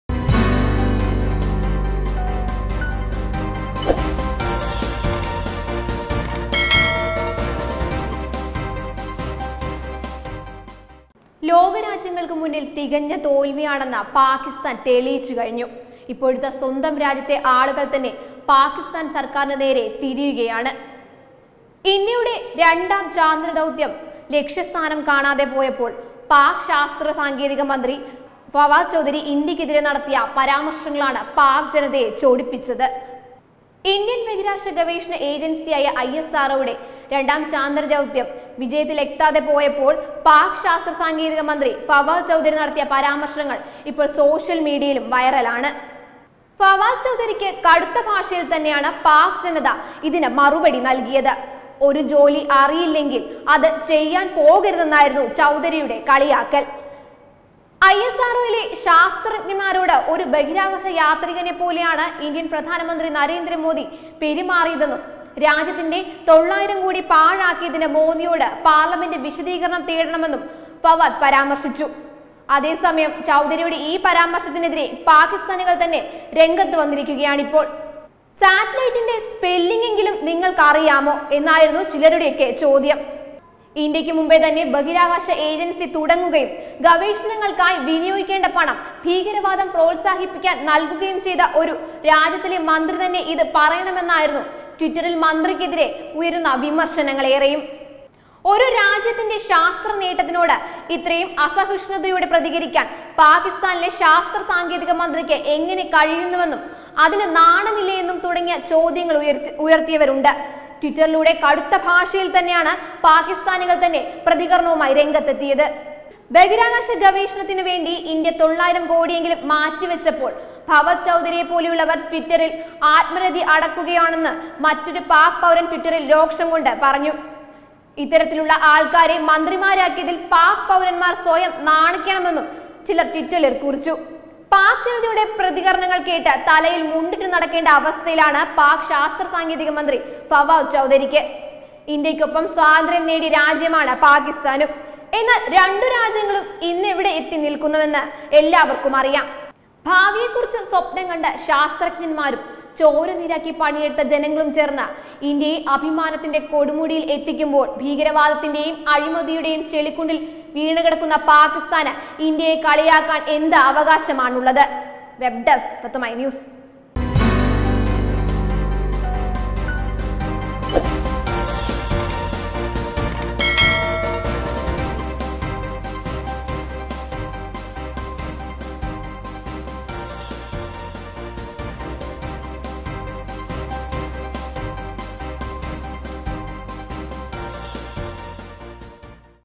സാറ്റ് ലൈറ്റിന്‍റെ സ്‌പെല്ലിംഗ് എങ്കിലും അറിയാമോ? പാക് ജനതയുടെ പ്രതികരണം ഞെട്ടിച്ചു
ലോക രാജ്യങ്ങള്‍ക്ക് മുന്നില്‍ തികഞ്ഞ തോല്‍വിയാണെന്ന് പാകിസ്ഥാന്‍ തെളിയിച്ചു കഴിഞ്ഞു. ഇപ്പോഴിതാ സ്വന്തം രാജ്യത്തെ ആളുകള്‍ തന്നെ പാകിസ്ഥാന്‍ സര്‍ക്കാരിന് നേരെ തിരിയുകയാണ്. ഇന്ത്യയുടെ ചാന്ദ്രദൗത്യം ലക്ഷ്യം കാണാതായപ്പോള്‍ പാക് ശാസ്ത്രസാങ്കേതിക മന്ത്രി ഫവാദ് ചൗധരി ഇന്ത്യക്ക് എതിരെ
നടത്തിയ പരാമര്‍ശങ്ങളാണ് പാക് ജനതയെ ചൊടിപ്പിച്ചത്.